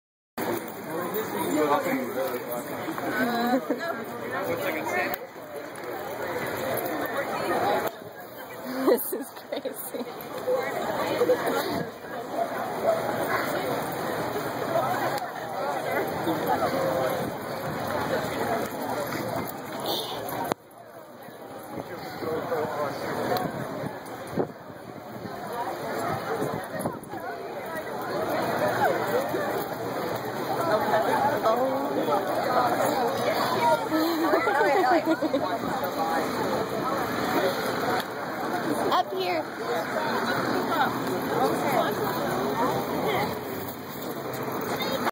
Corgi Walk in the Pearl 2012 - Portland, OR
Hundreds of Corgi's and their kibble givers take a mile stroll through downtown Portland.